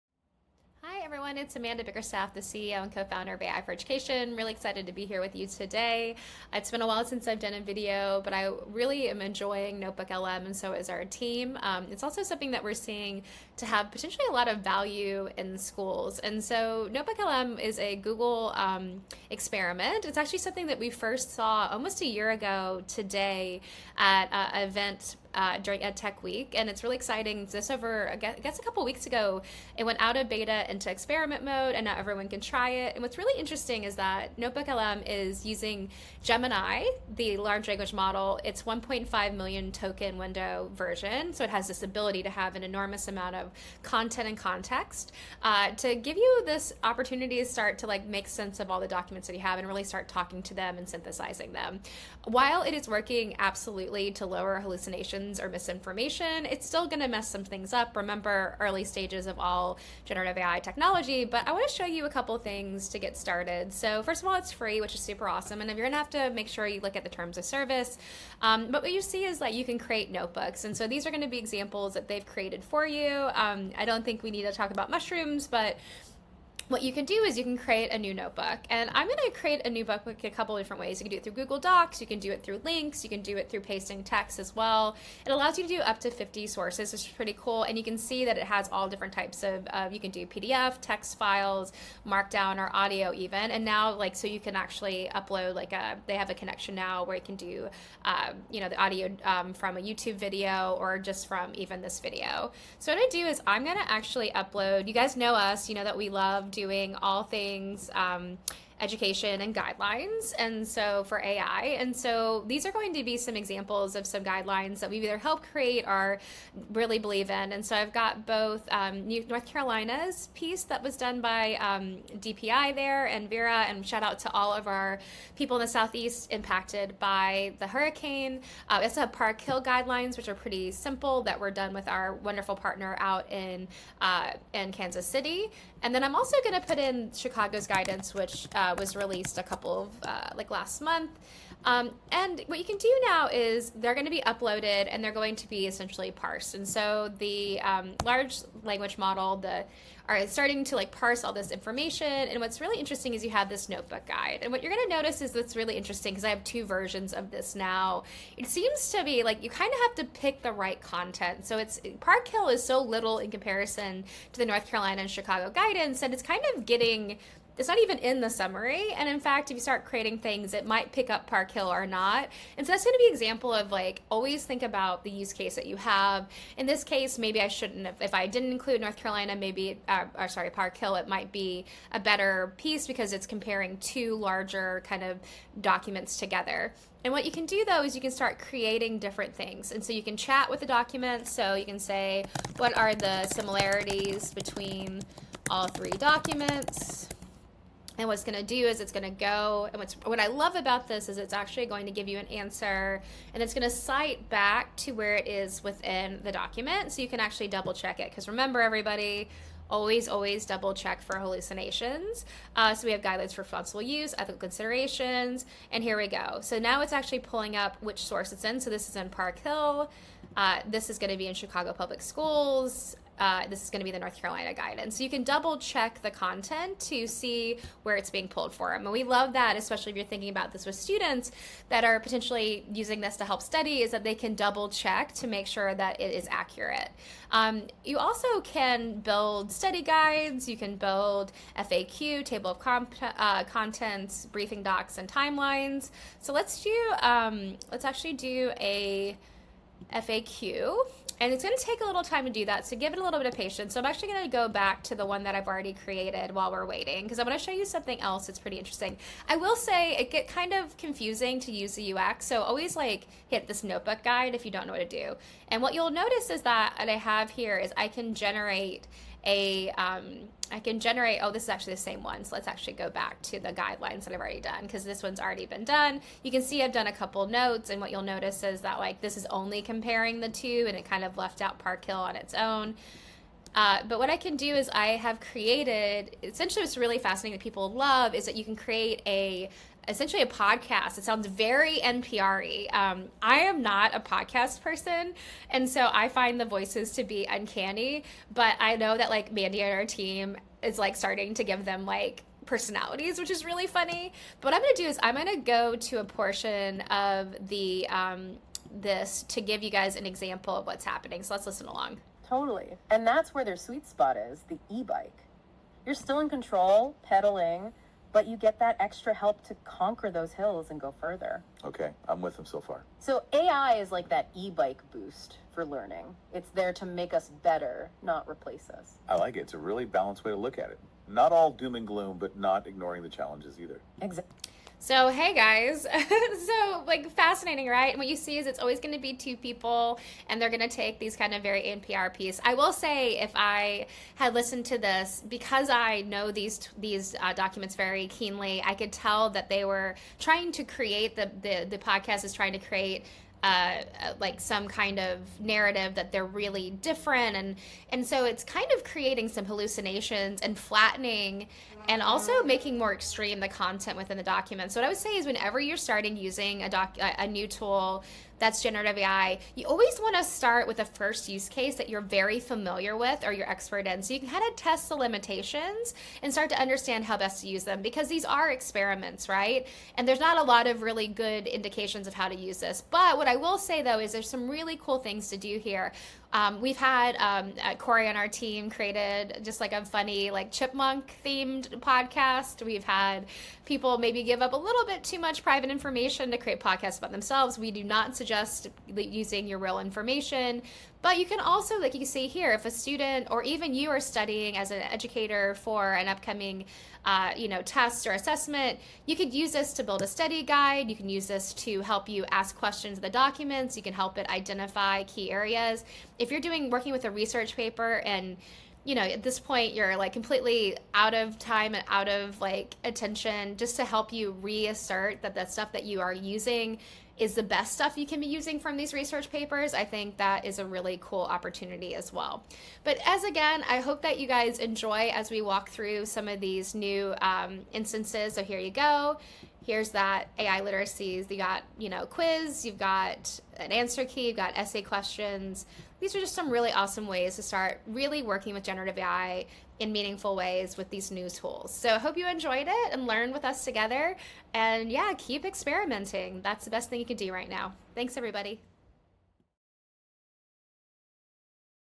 Notebook LM Review
Product demo and review of Notebook LM by Amanda Bickerstaff, CEO of AI for Education providing an overview of this innovative tool that leverages the Gemini LLM to facilitate extensive content management with a 1.5 million token window.

Amanda showcased key features for educators, including the ability to consolidate information from up to 50 different sources—such as Google Docs, PDFs, and text files—into organized notebooks. Notable functionalities were demonstrated, such as summarizing uploaded documents, and a chat feature that cites sources in responses, ultimately aiding in the creation of study aids like quizzes and FAQs.

Amanda also reviewed the podcast generation feature, which produces dialogues in an NPR-style format but comes with a warning regarding potential inaccuracies.

Check out the whole review for more!